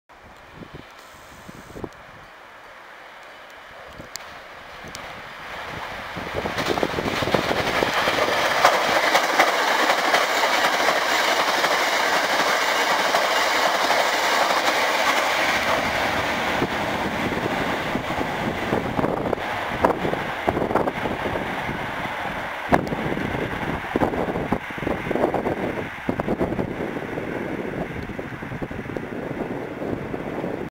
2017.01.16 阪急 9300系9303F 特急河原町行き通過 相川駅 阪急電鉄 阪急電車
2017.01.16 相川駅
9303F 8両編成
(Mc1)9303-(T1)9853-(T2)9873-(T2)9883-(T2)9893-(T1)9863-(M1)9803-(Mc2)9403